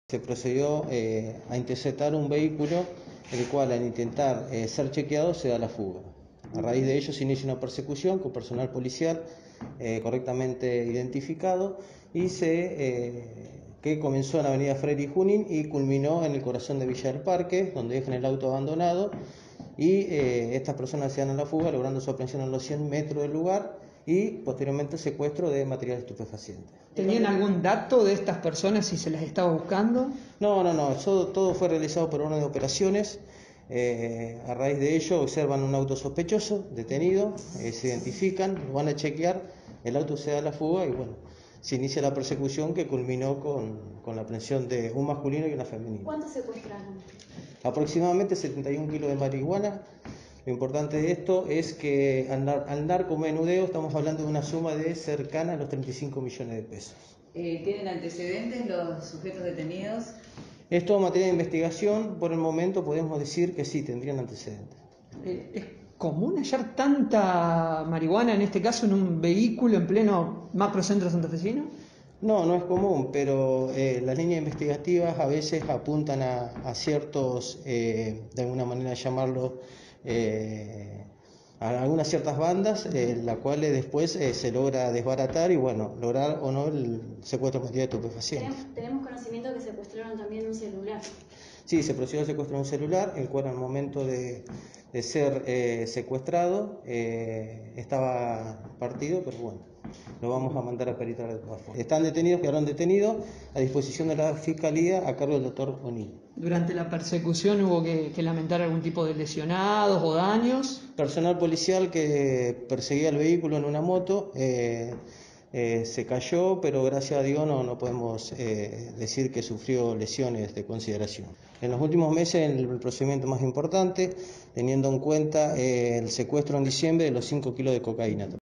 PERSECUCIÓN, DOS DETENIDOS Y 70 KILOS DE MARIHUANA SECUESTRADA
CRISTIAN LEMOS – AGENCIA DE INVESTIGACIÓN CRIMINAL